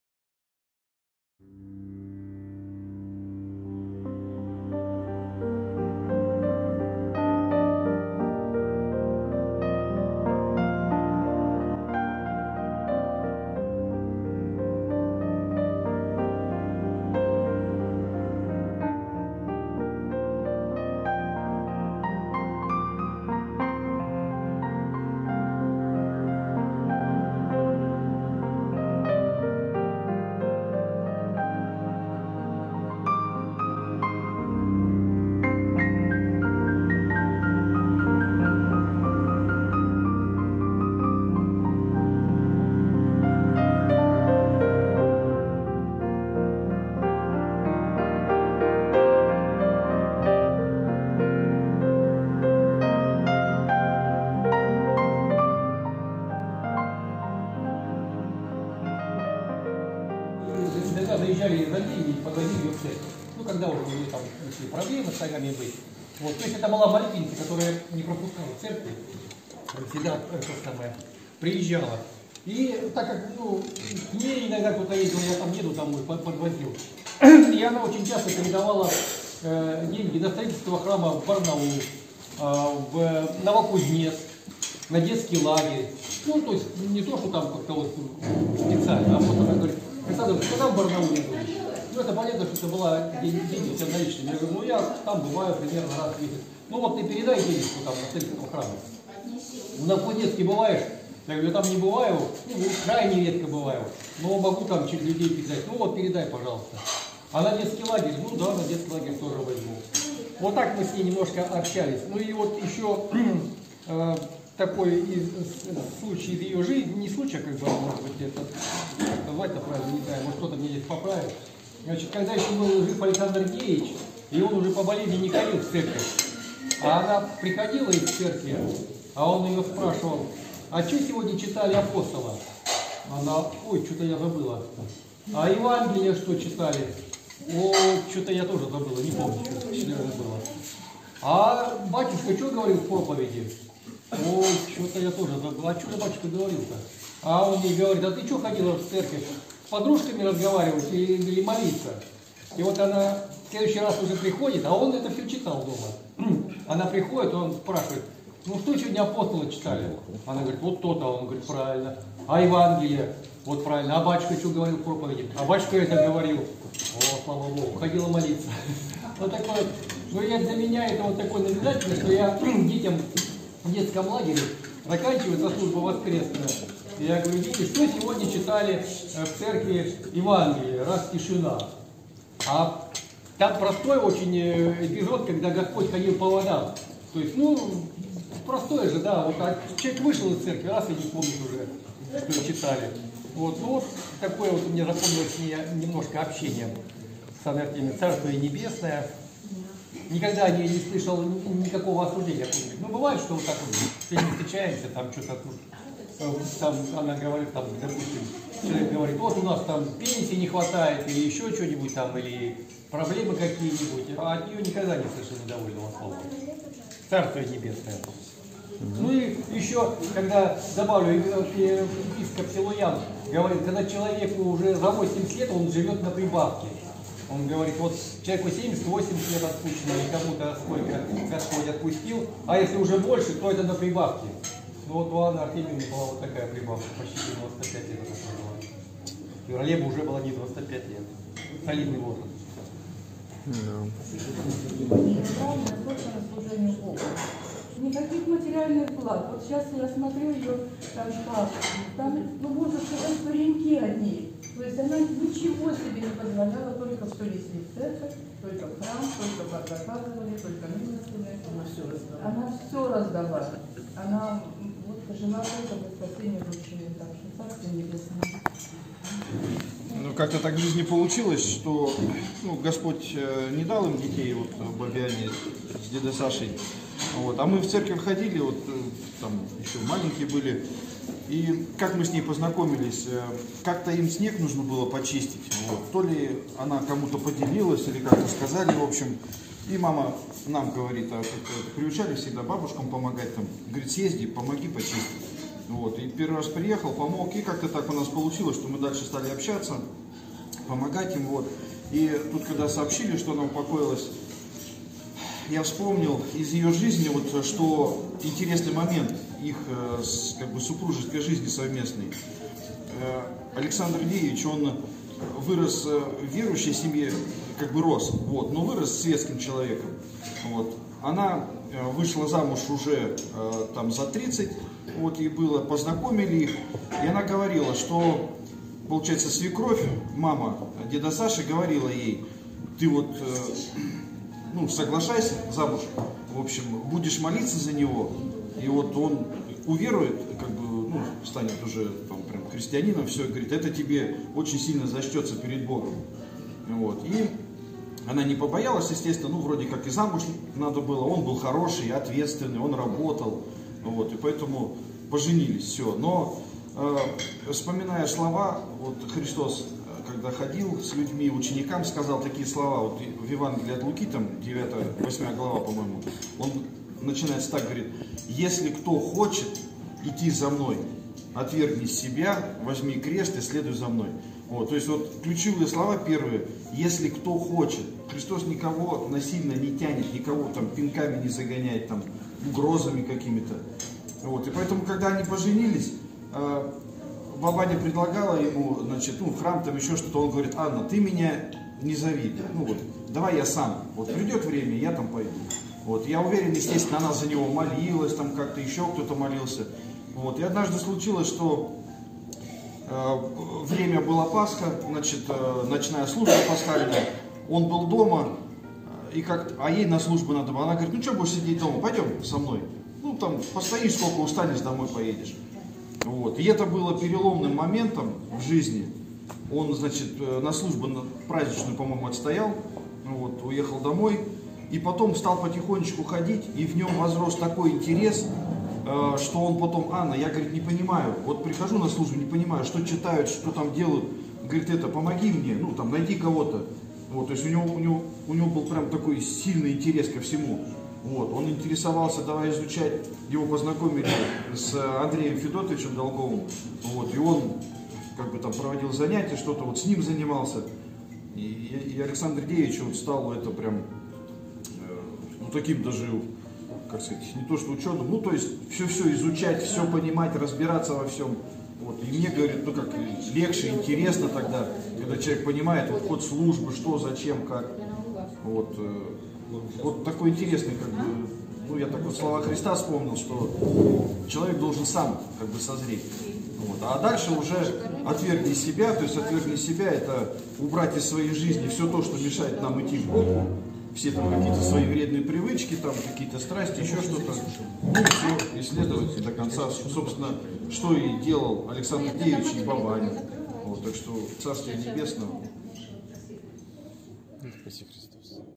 95 лет было Ситниковой Анне Артемьевне. Проводили в последний путь и вспомнили её добрым словом 🙏🏻
Бабушка Аня прожила почти 95 лет. Она выросла в христианской семье и была верующей христианкой до самого последнего дня жизни. На её похоронах делились разными воспоминаниями из жизни бабушки Ани. Бабушка Аня всегда поминала усопших православных христиан молитвою и милостынею.